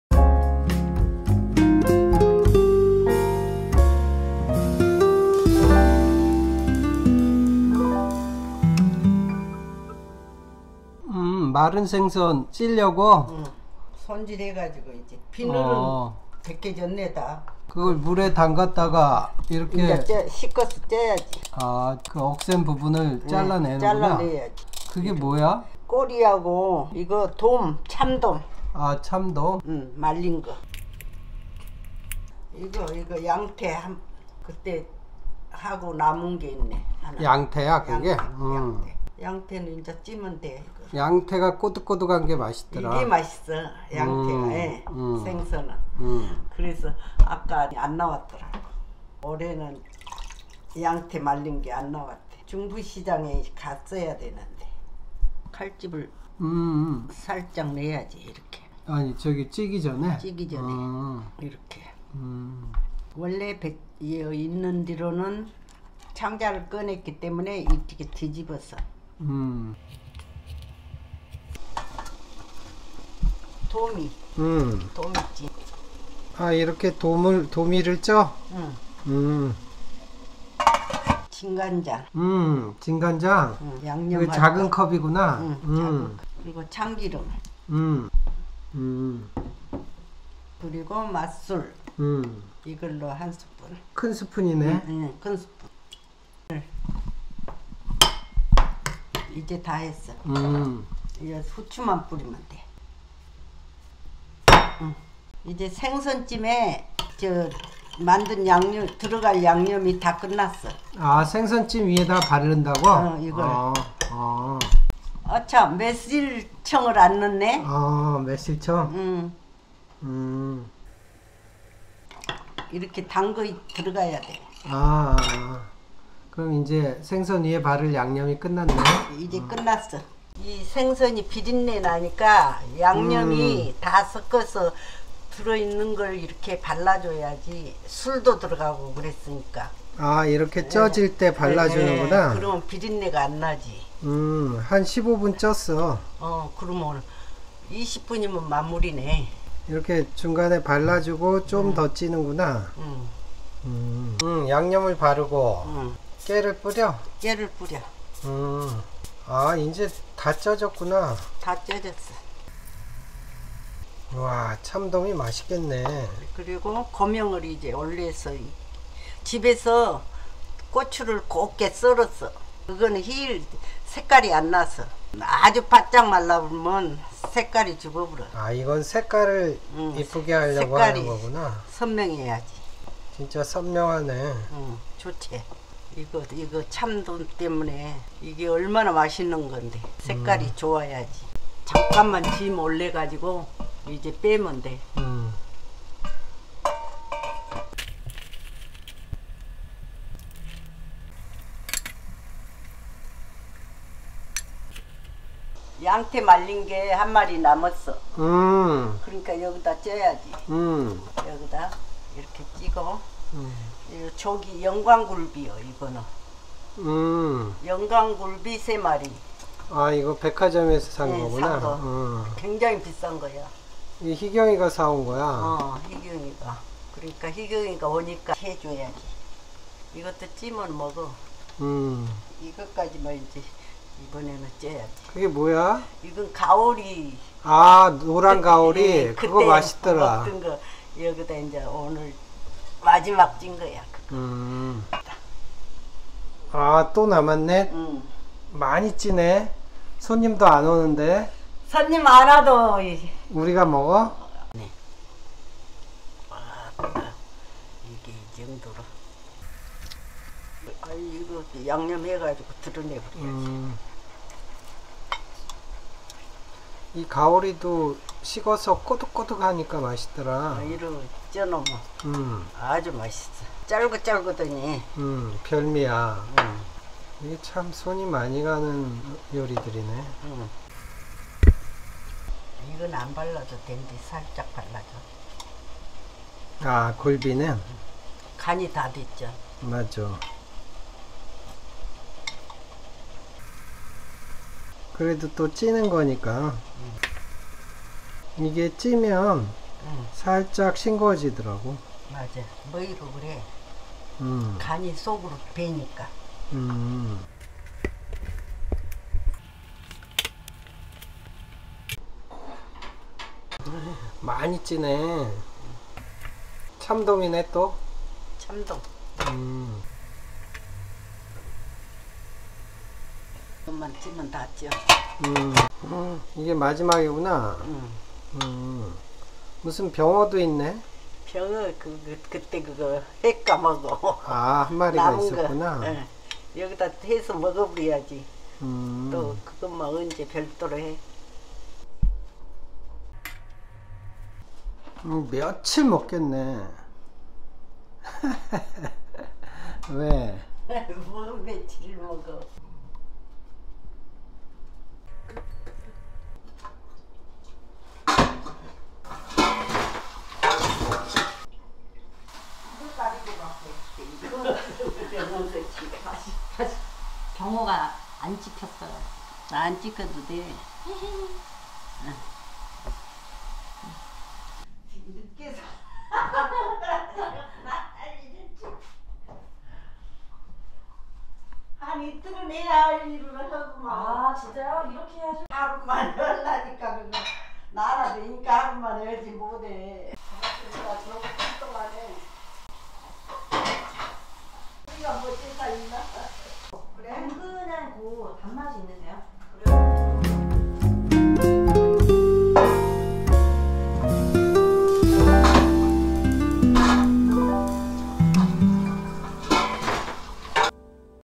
반건조 생선찜/말린생선 양념장 만들기
참돔,양태,굴비,가오리를 말려서 찜요리를 만들었어요.
잘마른 생선찜요리는 식감도 맛도 뛰어나네요.
[마른 생선찜]

*재료
참돔 17마리
양태 1마리
노랑가오리 1마리
굴비 3마리
병어 2마리
매실청 15cc
진간장 1작은컵=60cc
참기름 5cc
맛술 15cc
마늘 1스푼
후추 1스푼
실고추 3꼬집
깨 5스푼

*만드는 법
1.잘마른 생선의 날카로운부분들은 가위로 다듬어준다.
2.찜통에 넣고 15분간 쪄준뒤 양념을 바르고 5분간 더쪄주면 찜완성
3.진간장에 참기름,맛술을 넣어준다.
4.다진마늘 또는 즙을 넣고 후추를 넣은후 잘섞어주면 양념장완성
5.다쪄진후 실고추를 뿌리고 깨도 뿌려준다.